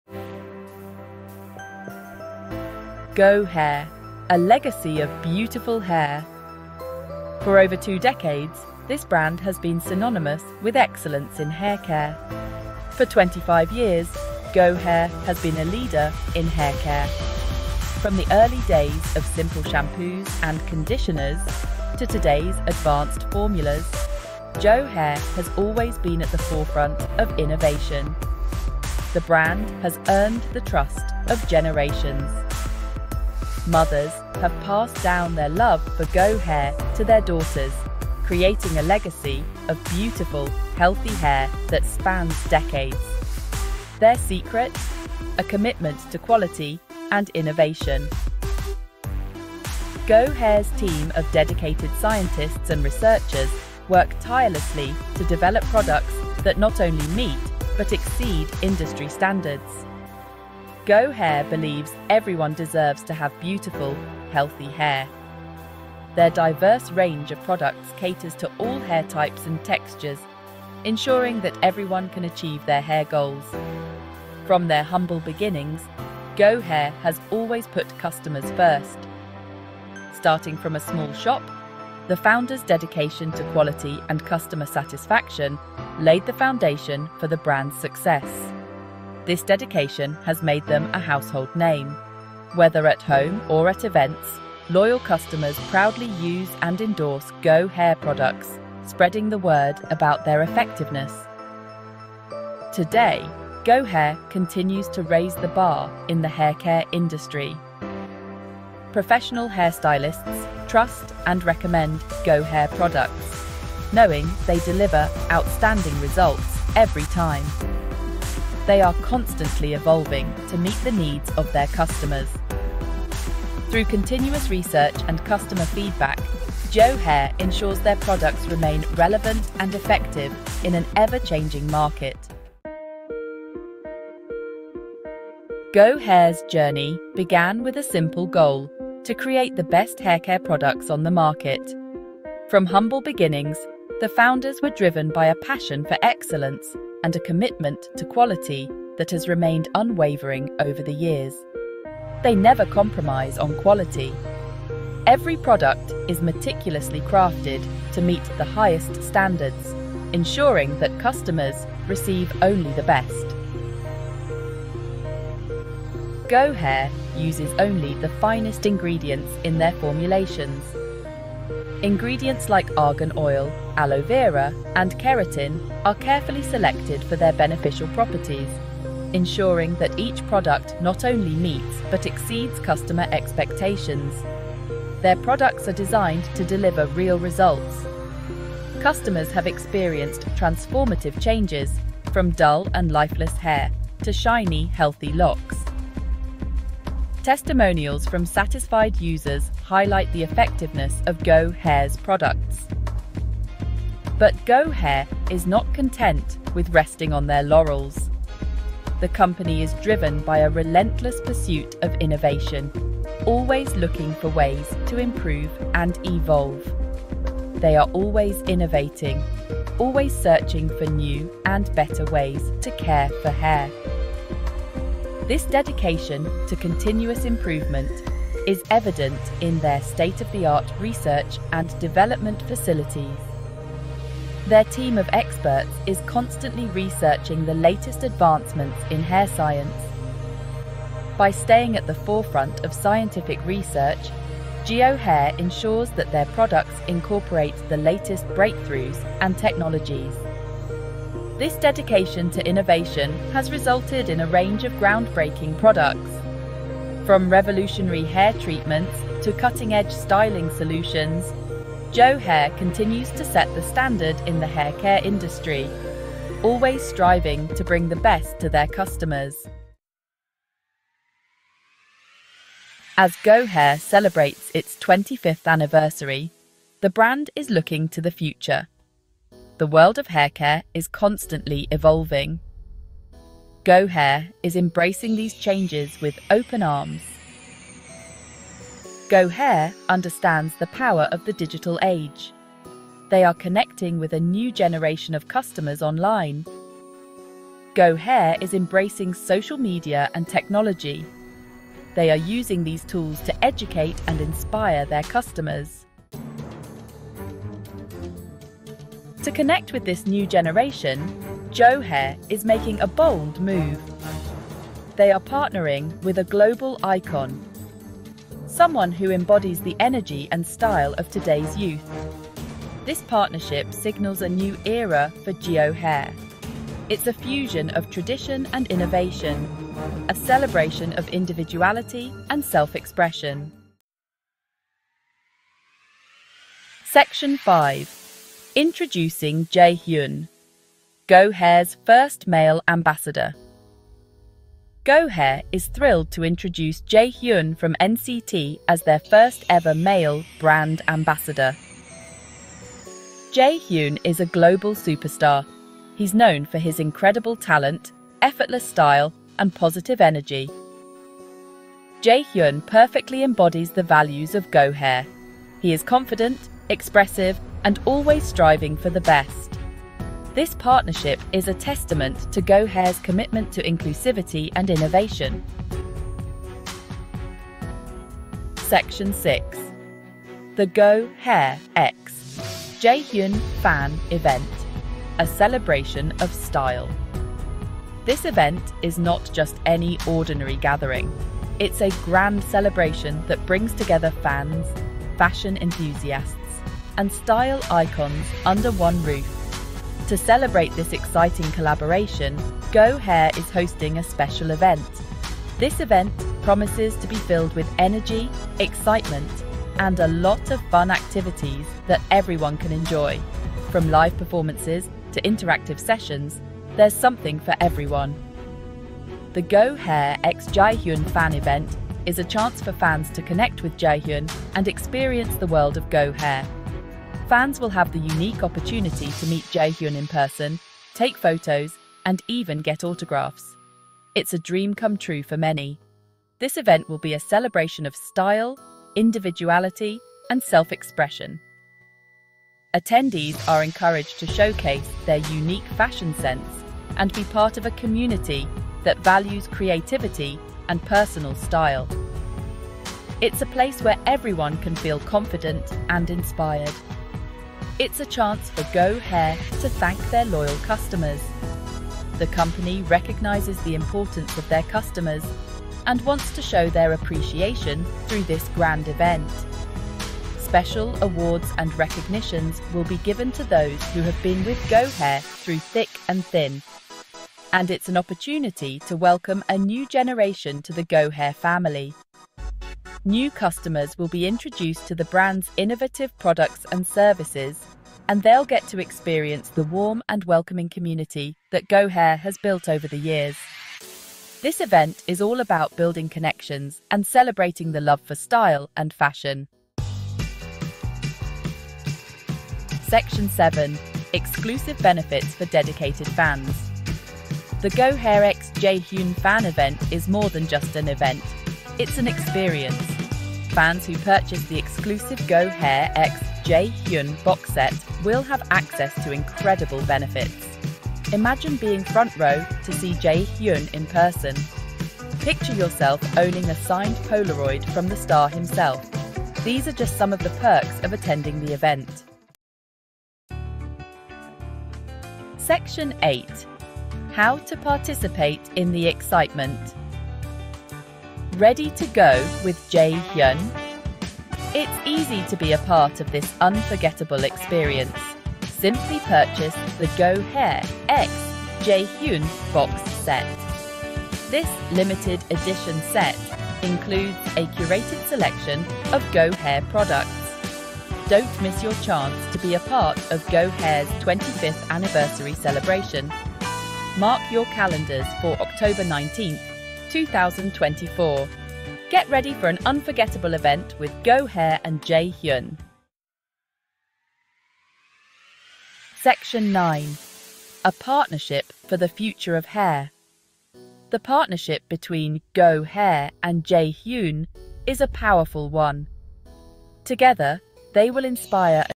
"GO HAIR Celebrates 25 Years with JAEHYUN NCT: A New Era in K-POP! "
OUTLINE:
00:00:00 A Legacy of Beautiful Hair
00:02:12 Twenty-Five Years of Quality and Innovation
00:04:29 Embracing the Future with Open Arms
00:05:06 A New Generation, A New Ambassador
00:05:33 GO HAIR's First Male Ambassador
00:06:15 A Celebration of Style
00:08:19 Exclusive Benefits for Dedicated Fans
00:08:52 How to Participate in the Excitement
00:09:35 A Partnership for the Future of Hair
00:10:14 Join GO HAIR and JAEHYUN on This Exciting Journey

**Category:**
Entertainment

**Description (Thai):**
🎉✨ สวัสดีค่ะทุกคน! วันนี้เรามาเฉลิมฉลองความสำเร็จของ GO HAIR ในโอกาสครบรอบ 25 ปี! 🎊🎈 พบกับ JAEHYUN NCT ที่จะมาเป็น Brand Ambassador คนแรกของเรา! 💖👦🏻 เขาคือ All-rounder แห่งวงการ K-POP ที่จะนำพา GO HAIR ก้าวเข้าสู่ New Era 🌟🌍

ในคลิปนี้เราจะพูดถึงความพิเศษของผลิตภัณฑ์ GO HAIR และการเปลี่ยนแปลงที่น่าตื่นเต้นในอนาคต! 🔥💇‍♂️ รวมถึงเบื้องหลังการถ่ายทำและความคิดของ JAEHYUN เกี่ยวกับแบรนด์ของเรา 💬💕

**Description (English):**
🎉✨ Hello everyone! Today, we are celebrating GO HAIR's amazing journey as we mark our 25th anniversary! 🎊🎈 Join us with JAEHYUN NCT, who will be our very first Brand Ambassador! 💖👦🏻 He is the all-rounder of the K-POP industry, leading GO HAIR into a New Era! 🌟🌍

In this video, we will discuss the special features of GO HAIR products and the exciting changes that lie ahead! 🔥💇‍♂️ Plus, get an exclusive behind-the-scenes look at the shoot and JAEHYUN's thoughts on our brand! 💬💕